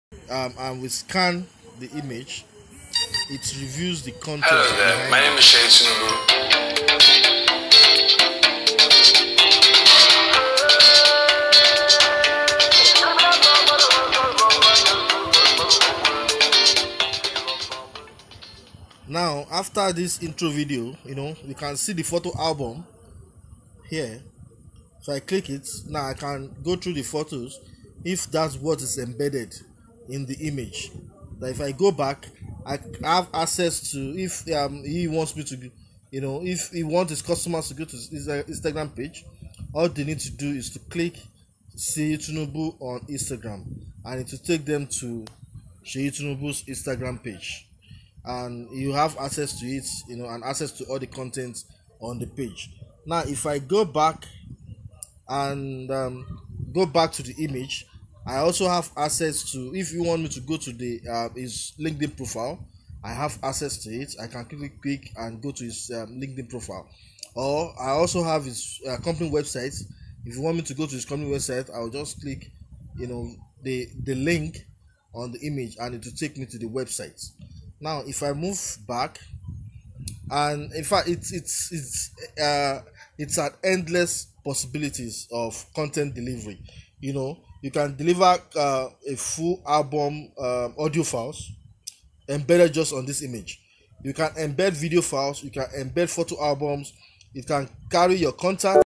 Augment Everything
This is an Augmented Reality demo video showing some of the use cases of what's possible with AR and what the future holds